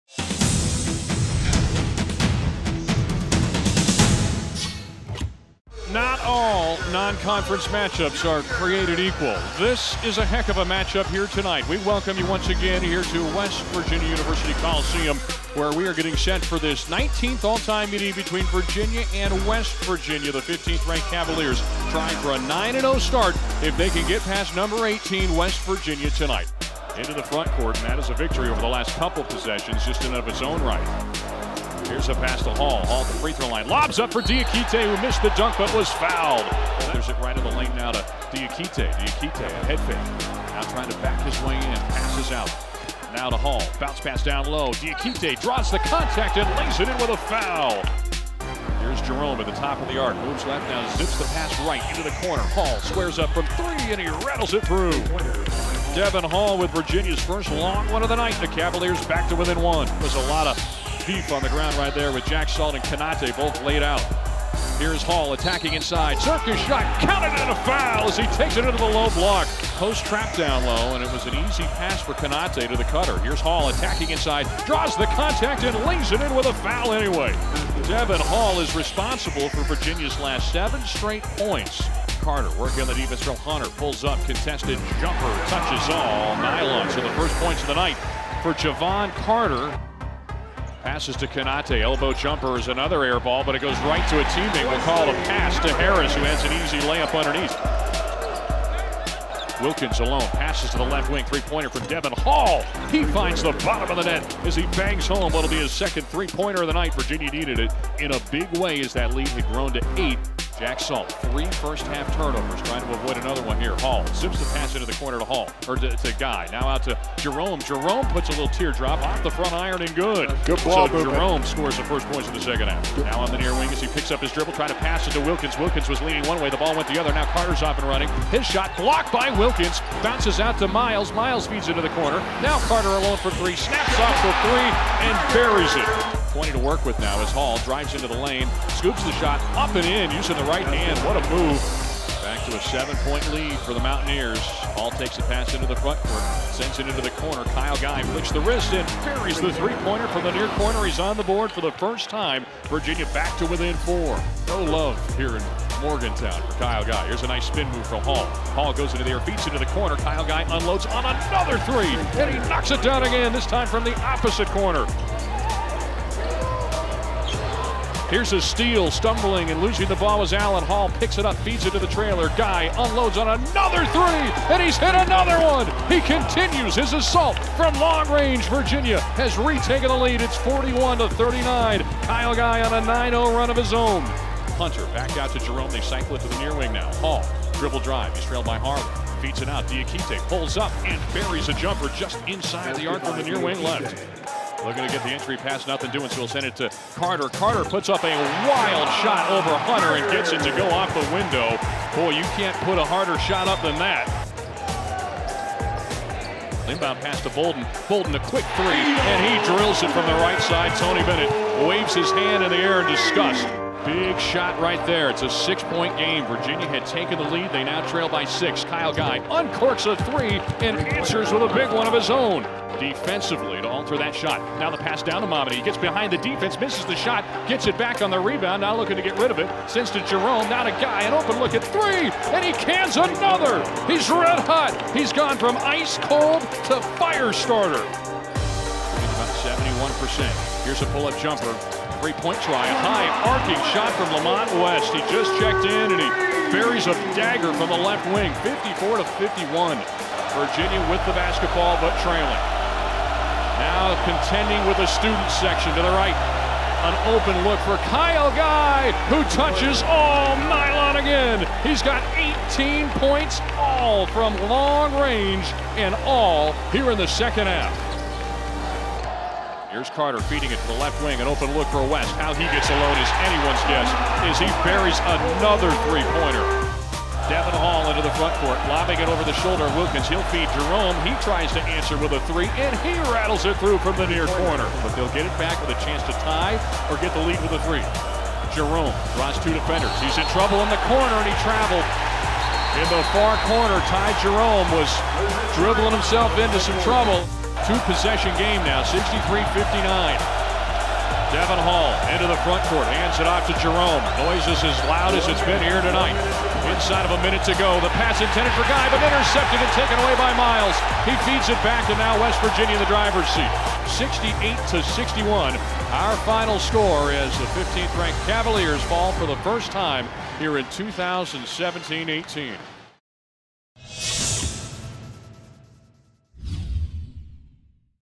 MEN'S BASKETBALL - WVU Highlights
'Hoos drop their first game of the season in Morgantown.